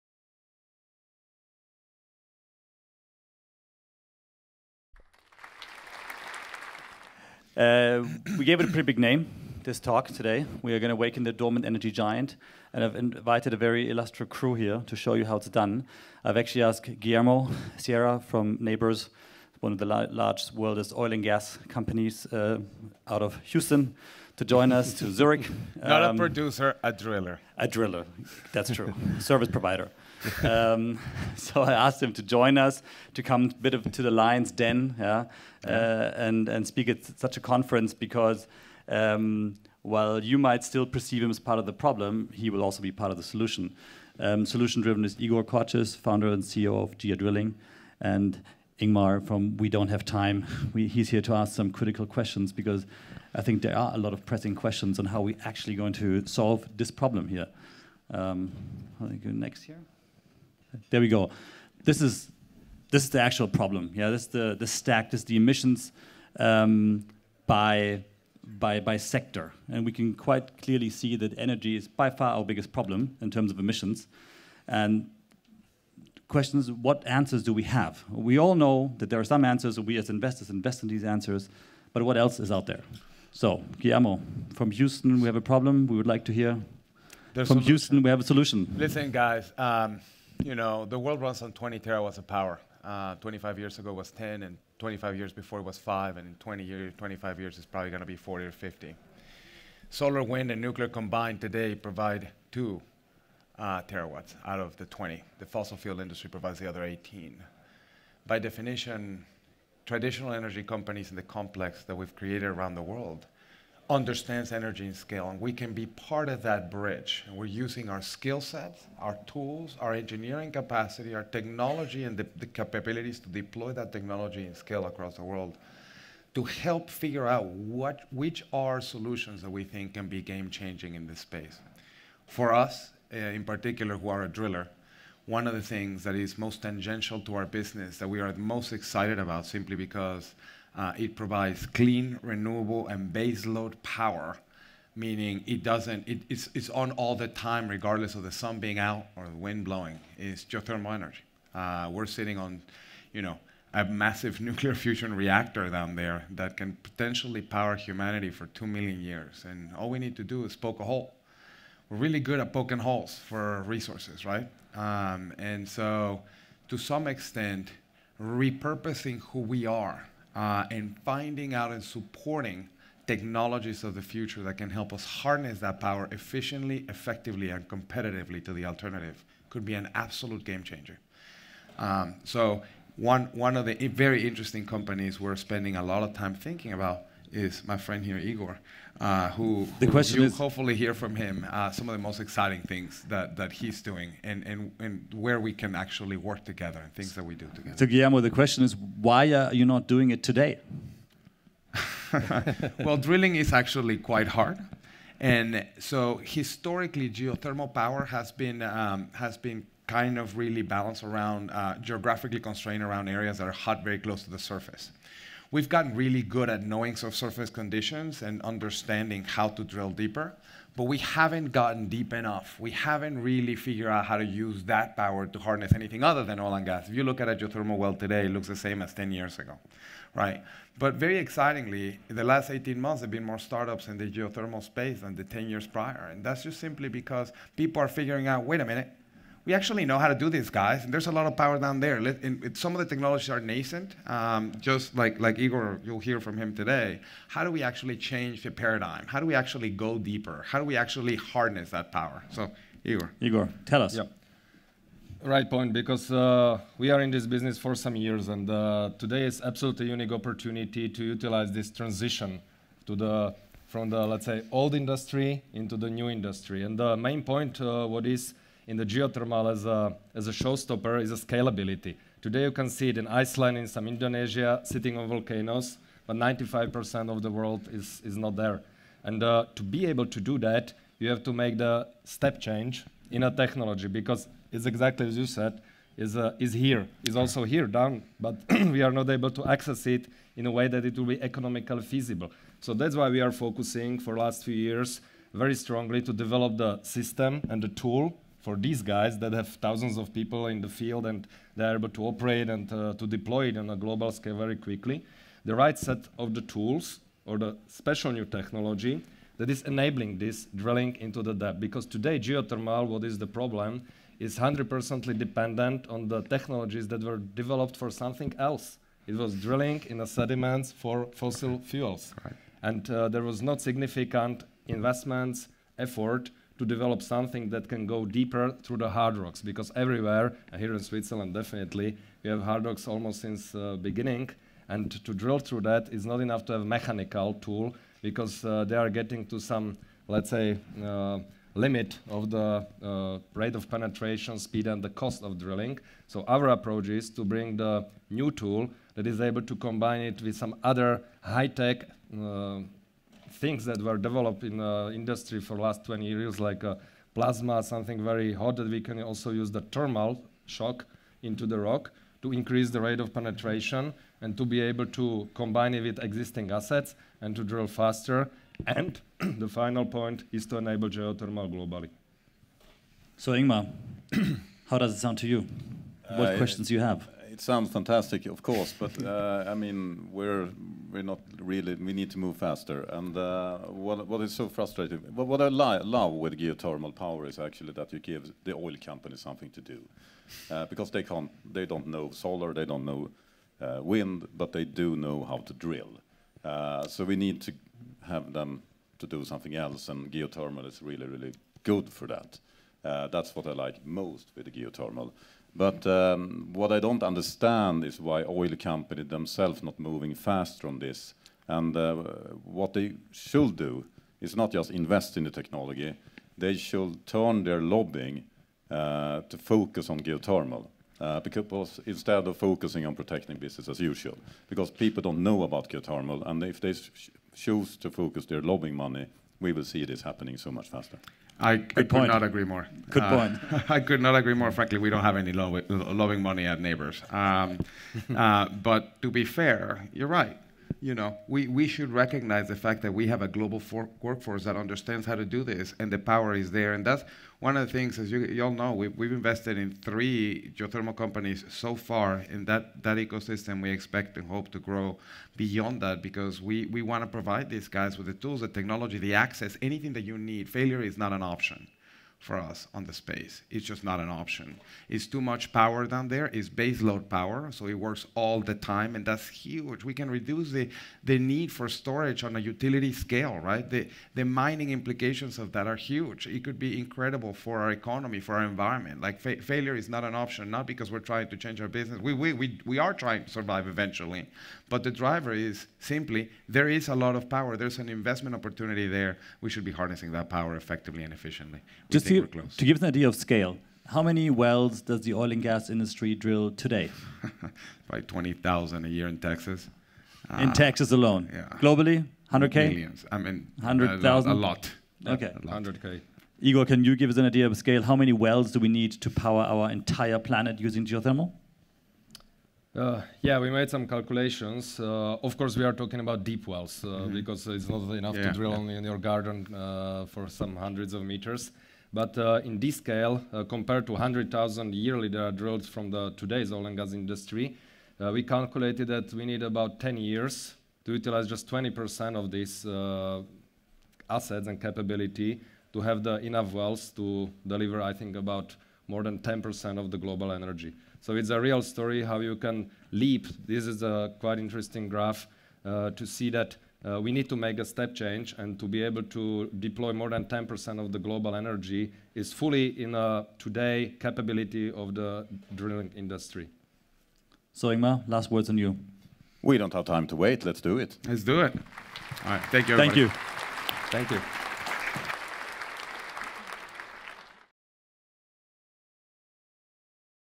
Awakening the Dormant Energy Giant - NOAH21 Zurich
Panel with Guillermo Sierra, VP Strategic Initiatives - Nabors - Igor Kocis, Founder & CEO - GA Drilling - Sebastian Heitmann, Co-Founder - Extantia and Ingmar Rentzhog, Founder & CEO - We Don't Have Time at the NOAH Conference Zurich 2021, 6-7 December, The Circle.